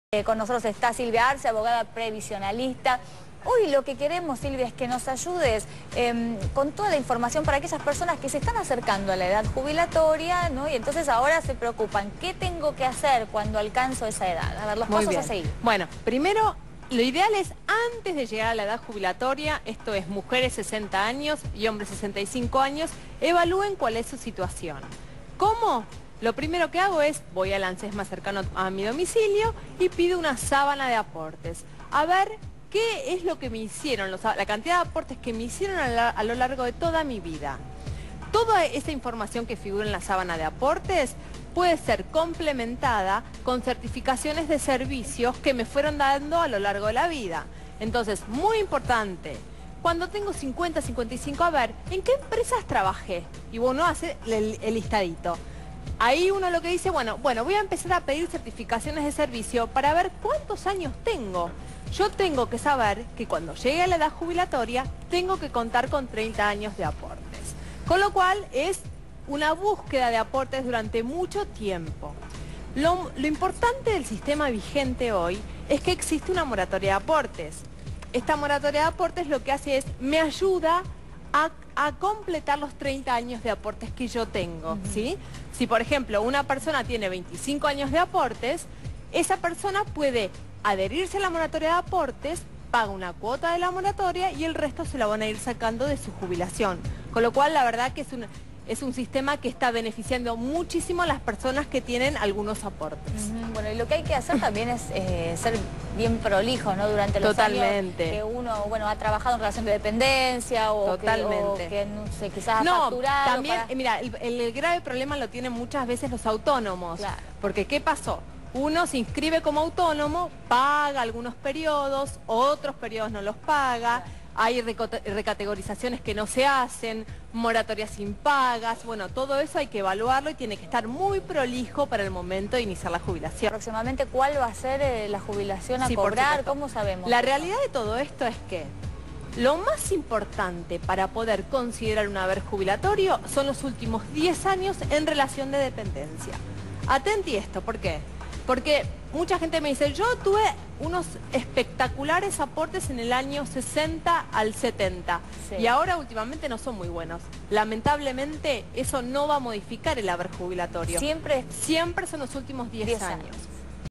Los pasos a seguir para acceder a una jubilación
En el canal 26, la especialista en el área previsional Silvia Arce explica cómo acceder a una jubilación cuando se está cerca de la edad mínima jubilatoria.